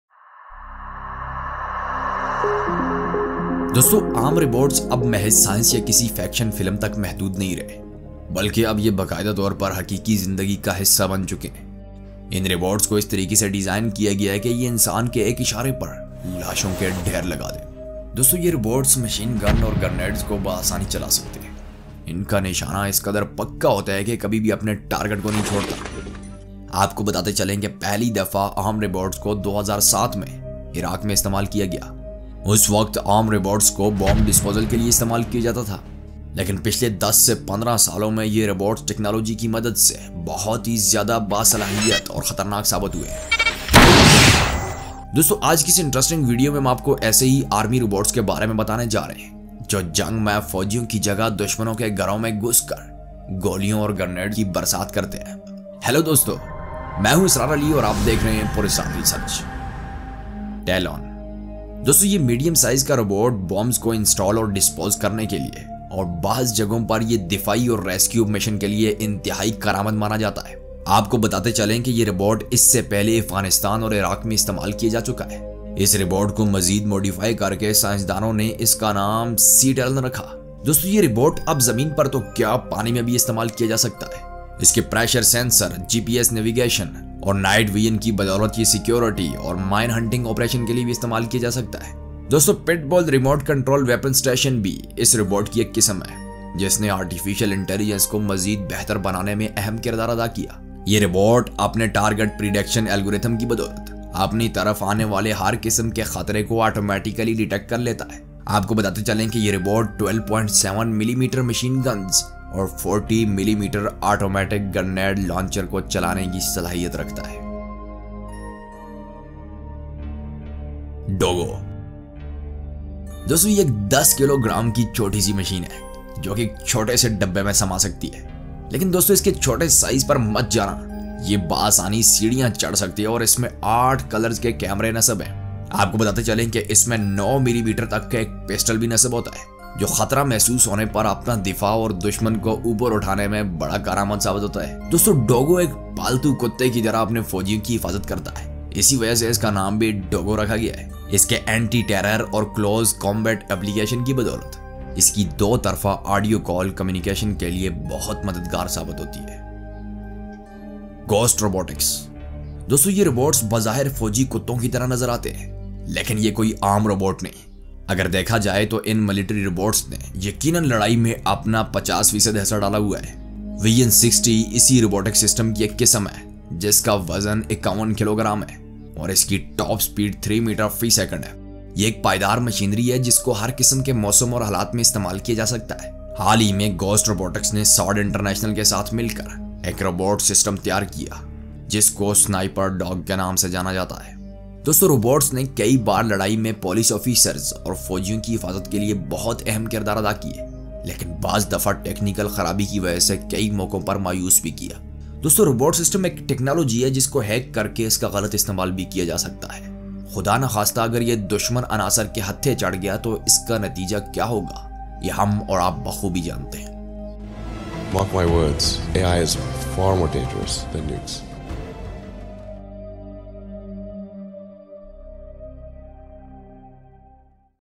3 Future Killer Robots That Will Change The World
Friends, armed robots are no longer limited to science or a fiction film, but now they have officially become a part of real life.
These robots are designed in such a way that they can pile up corpses at a human's signal.
These robots can operate machine guns and other weapons with ease and their aim is so precise that they never miss their target.
Friends, let me tell you that for the first time, armed robots were used in Iraq in 2007.
At that time armed robots were used for bomb disposal but now
 In the last 10 to 15 years, these robots have proven to be capable and dangerous.

3 Future Killer Robots That Will Change The World